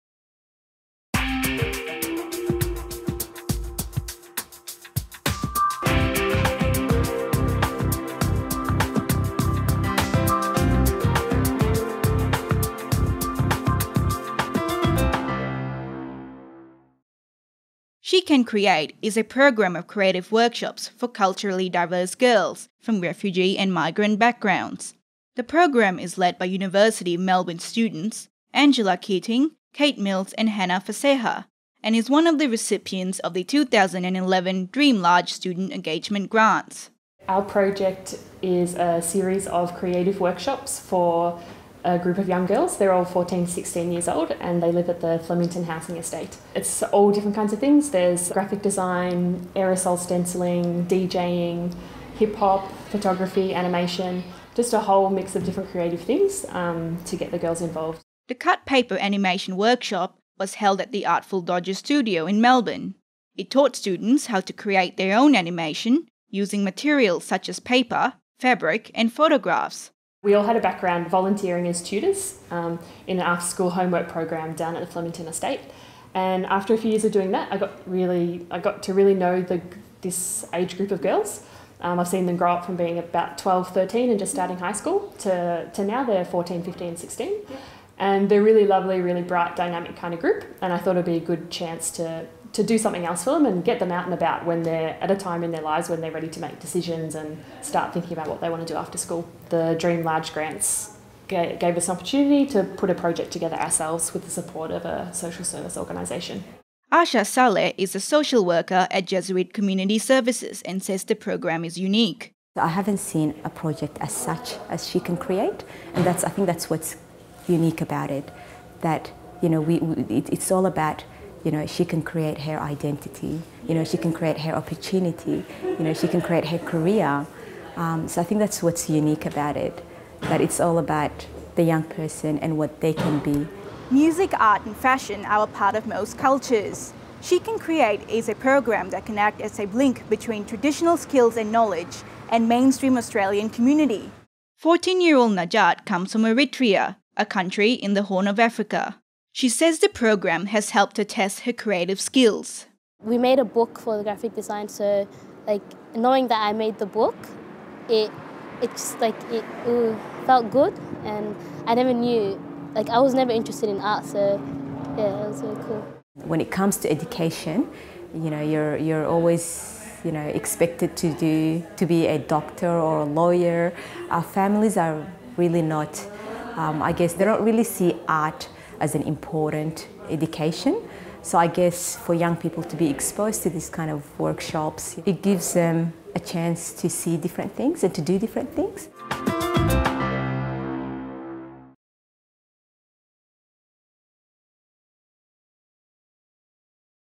Visions: She Can Create
She Can Create, a program led by undergraduate students from the University of Melbourne, is a series of creative workshops for culturally diverse girls from refugee and migrant backgrounds.  Visions tags along to an animation workshop to see how it all works.

You can find further images from the She Can Create program on Facebook

The program was made possible thanks to a Dreamlarge Student Engagement grant. More information on these grants can be found

Produced by Navina Smith
Camera by Clive Banfield
Editing by Robert Cross
Additional stills provided by Angela Keating
Additional video provided by Kate Matthews

With special thanks to Angela Keating, Asha Saleh, Kate Mills, Hannah Fesseha and Kate Matthews.